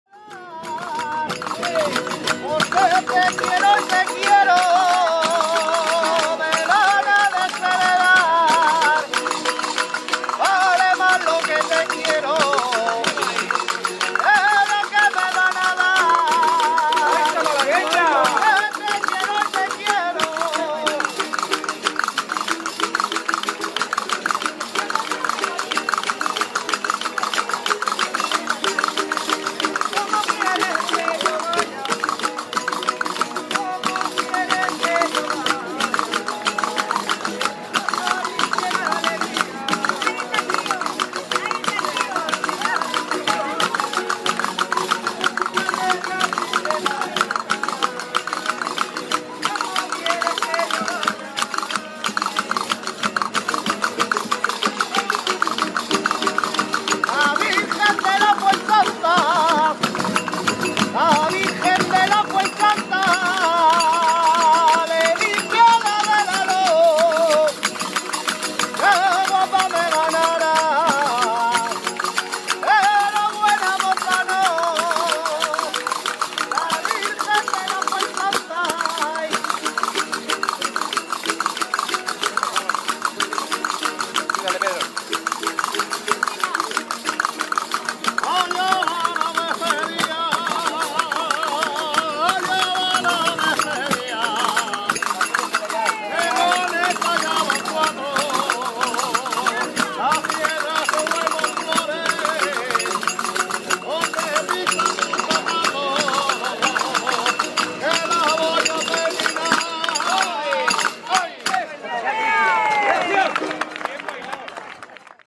Barranda 2018. Cuadrilla de Patiño. Malagueña. Barranda (Murcia, España), 28-01-2018
La Fiesta de las Cuadrillas de Barranda (Caravaca, Murcia, España) celebró su 40 edición en 2018 y se ha consagrado como un festival de referencia sobre músicas de tradición oral hispanas, entre otros factores (como las actuaciones de grupos folk o las jornadas y talleres sobre cultura popular) debido a su decidida apuesta por facilitar la participación popular y mostrar a los grupos que acuden en plena interacción con el público asistente, difuminando en lo posible la separación entre actores y espectadores y soslayando las barreras físicas que suele haber en otros encuentros musicales.
 Así, durante el último fin de semana de enero de cada año, en Barranda se dan cita numerosos aficionados de diversas procedencias para compartir músicas y vivencias, y poder disfrutar con los  diversos grupos y personas asistentes.
 A pesar de que la noche anterior al día del festival de esta 40 edición, el domingo 28 de enero, el tiempo meteorológico amenazó con dificultarlo con una nevada, en la mañana de dicho día despejó lo suficiente como para que la organización decidiera que los grupos salieran informalmente a la calle para animar la jornada. Así, el público, aunque en menor número que otras ediciones debido a las alertas por el tiempo, pudo disfrutar de una mañana soleada junto a los muchos grupos participantes, venidos desde lugares como las Islas Canarias, Castilla-León, Comunidad Valenciana, Andalucía, Castilla-La Mancha y la misma Murcia, … En la actual Región de Murcia existen varias comarcas con músicas de tradición oral vivas y diferenciadas. Una de ellas es la comarca de la Huerta de Murcia, sita en el corredor del río Segura donde se encuentra la capital de la región (Murcia ciudad). Una pequeña pedanía de Murcia muy cercana a la capital es Patiño, lugar con una notoria trascendencia en las cuestiones del folclore musical desde hace bastantes décadas. Allí, la Cuadrilla de las Benditas Ánimas de Patiño desarrolla en los últimos años una importante actividad de mantenimiento de los tradicionales rituales animeros y auroros y la participación en los llamados encuentros de cuadrillas (entre los que el de Barranda es considerado el más importante). En el repertorio de la Cuadrilla de Patiño se incluyen las músicas para baile suelto características de la comarca huertana, fundamentalmente malagueñas (variedades de fandango) y jotas. Un ejemplo de ello la incluimos en la Colección Memorias de la Tradición

con imágenes de esta malagueña de arriba tocada y cantada por la cuadrilla el domingo 28 de enero de 2018 en Barranda, en uno de los habituales corros espontáneos que se forman en la Fiesta de las Cuadrillas, mientras en el centro baila, exhibiéndose de manera elegante, una pareja al estilo del Raiguero de Totana.
En el cante se emplean varias coplas, cantadas con la habitual libertad expresiva que a veces hace cambiar las letras sobre la marcha, y que tienen temática amorosa, galante, melancólica o religiosa, como suele ser habitual en el cante por malagueñas, que en situaciones de lucimiento público es poco dado, por lo general y salvo excepciones, a lo grosero u obsceno.

Porque te quiero y te quiero
me van a desheredar,
vale más lo que te quiero
que lo que me van a dar.

Cómo quieres que yo vaya
al jardín de la alegría
si se marchitan las flores
al ver la tristeza mía.

La Virgen de la Fuensanta
le dice a la de la Luz:
a guapa me ganarás
pero a buena moza no.

Allá va la despedía
que con esta ya van cuatro,
las piedras se vuelvan flores
donde pisan tus zapatos,
que la voy a terminar.

Se ruega citar la procedencia y comunicar posibles errores